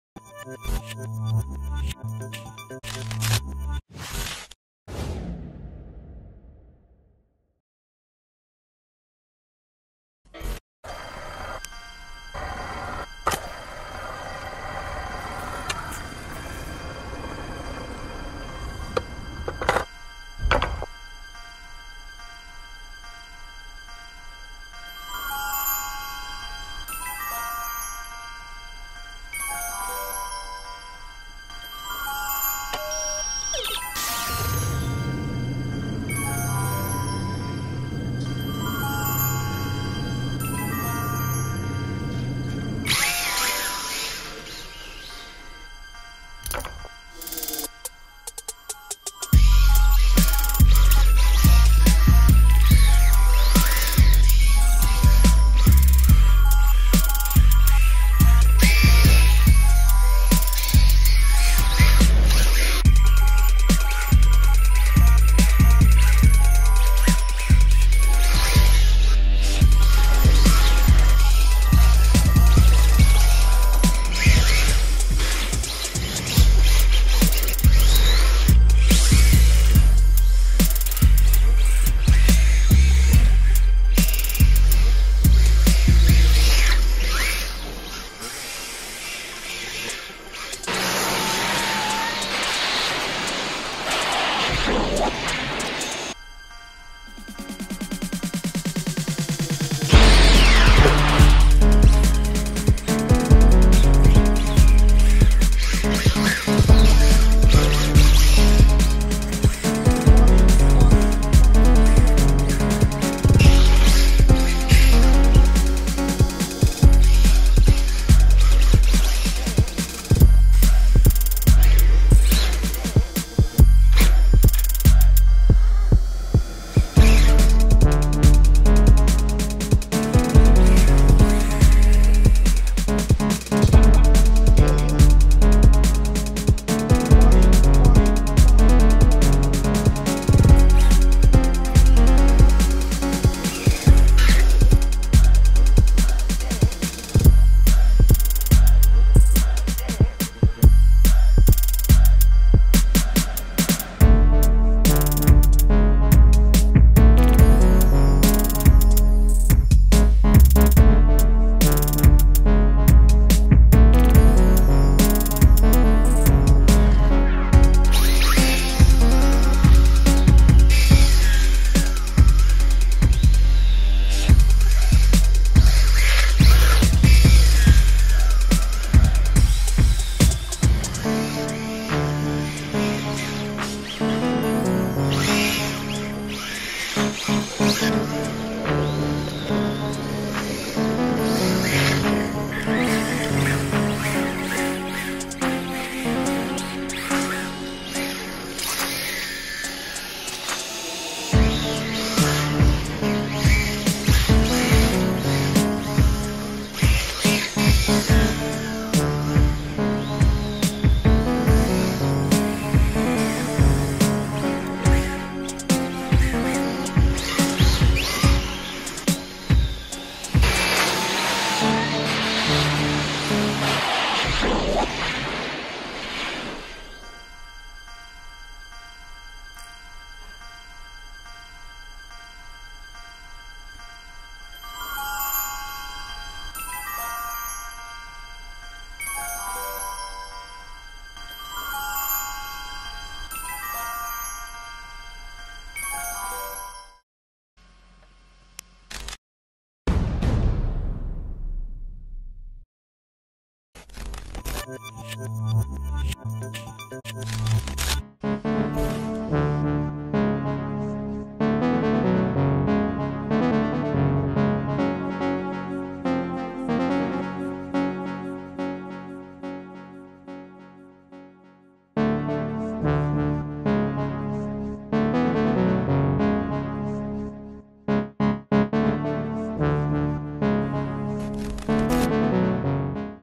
RC DRIFT CAR - 1:10 DELOREAN ride
This is the ride part from the build's video:
 -body: Delorean from Aplastic
 -Body: MST XXX-D